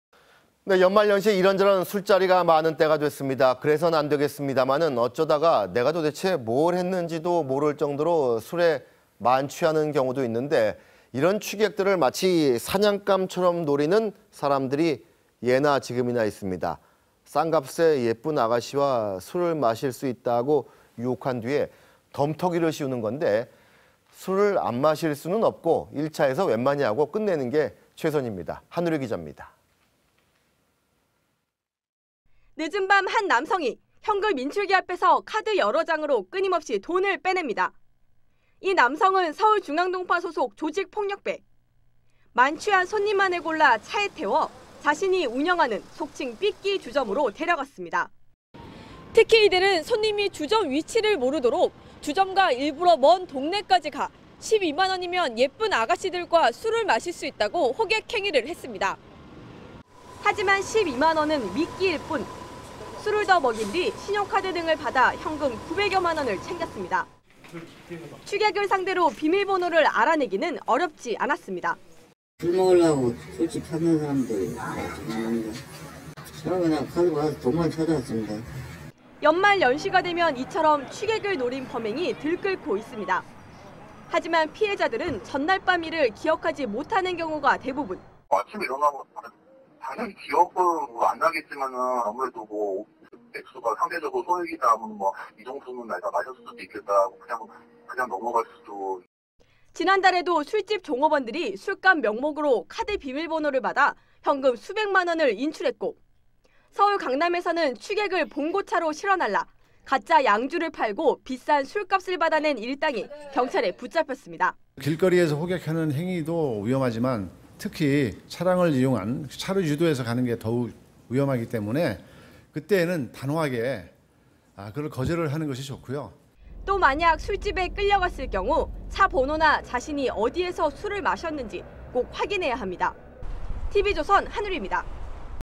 '12만원에 예쁜 여성과...' 삐끼의 유혹
연말 연시 이런저런 술자리가 많은 때가 왔습니다. 그래선 안 되겠습니다만 어쩌다가 내가 도대체 뭘 했는지도 모를 정도로 술에 만취하는 경우도 있는데 이런 취객들을 마치 사냥감처럼 노리는 사람들이 예나 지금이나 있습니다. 싼값에 예쁜 아가씨와 술을 마실 수 있다고 유혹한 뒤에 덤터기를 씌우는 건데 술을 안 마실 수는 없고 1차에서 웬만히 하고 끝내는 게 최선입니다.